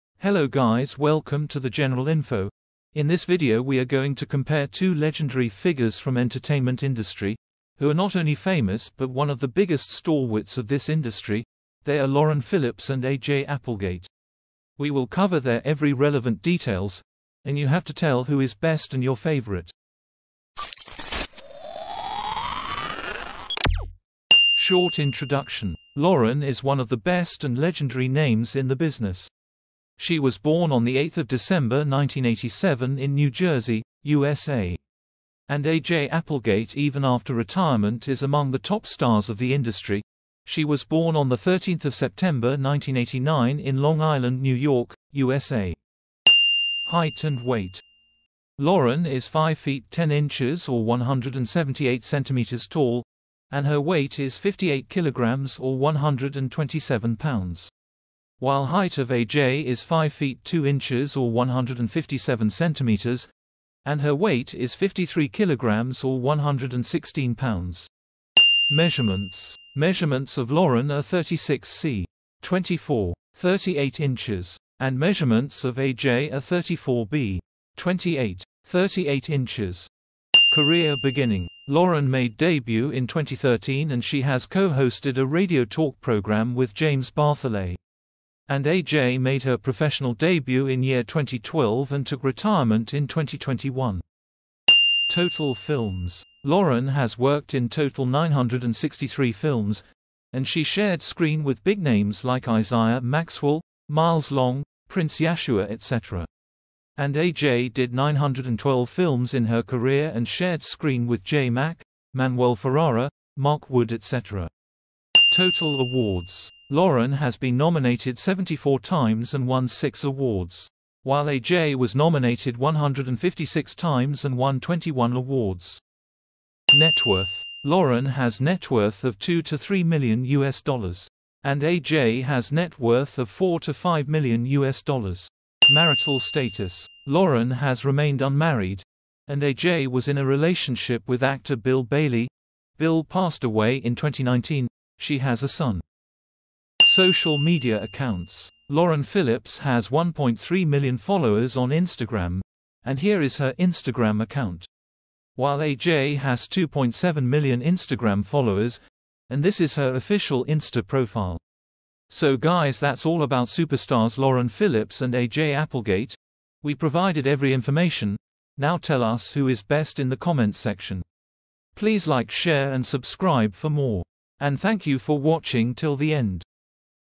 Lauren Phillips and AJ Applegate comparison | who is your favourite model?
Hello guys welcome to the general info.
in this video we are going to compare two legendary and the two best models of the entertainment industry, Lauren Phillips and AJ Applegate. from total films to awards we are showing everything. please watch this video till the end.